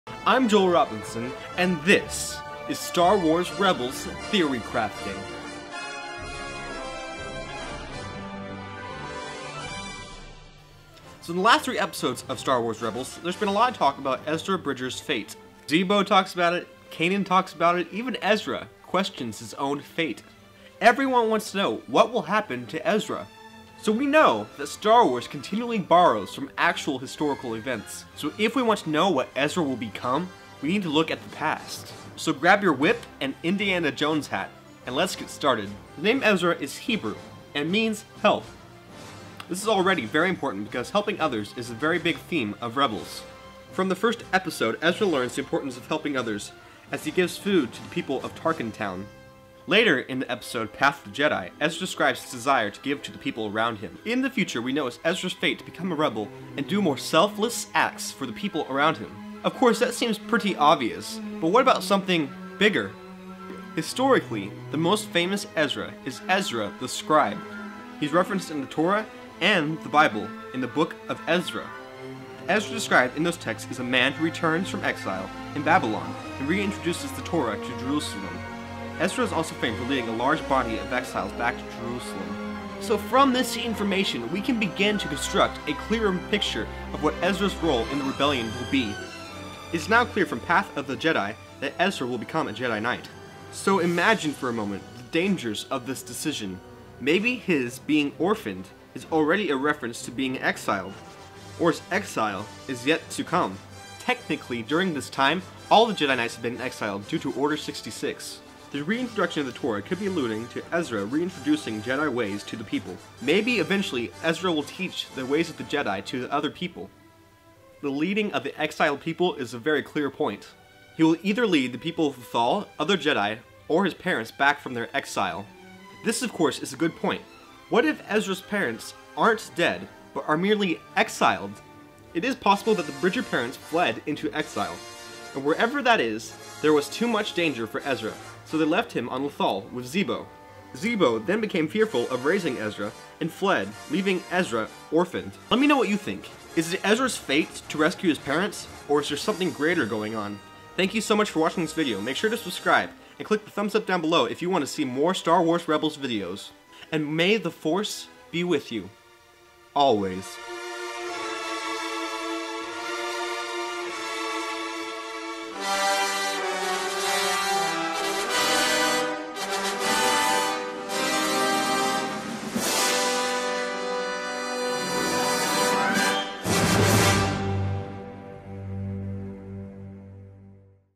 Ezra's Fate: Rebels Theorycrafting
This is my newest Star Wars Rebels theorycrafting video about Ezra Bridger's fate! Support me and get sweet rewards! -